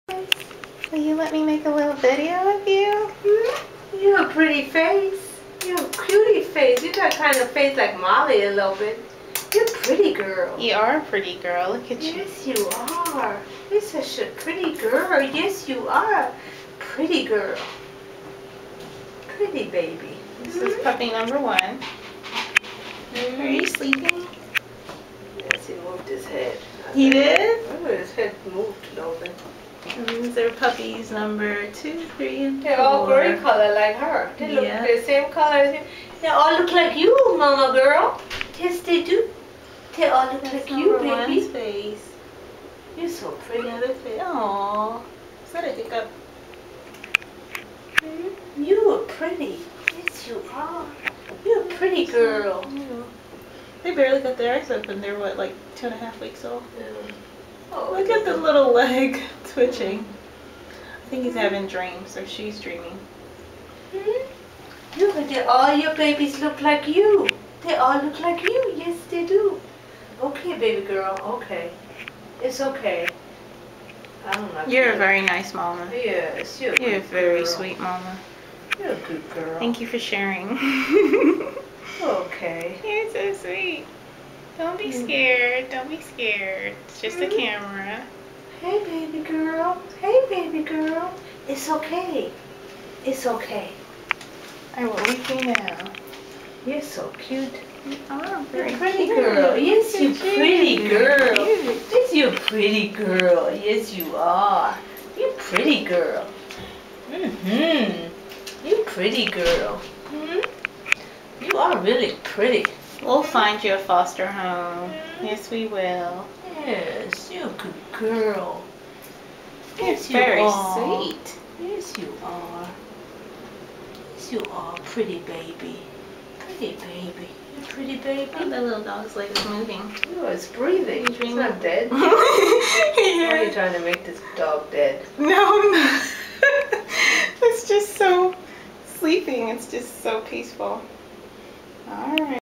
Sweet Momma Needs a Foster
Beautiful Gracie was rescued before she could be put down at the shelter.  She had 10 puppies about 2 weeks ago -- some have died and 3 are being bottle fed by a kind volunteer.  However, mom and her 4 healthy pups have nowhere to go right now.  They are in a room at our vet's office. Can you please foster them even if just for a weekend, a week, or two?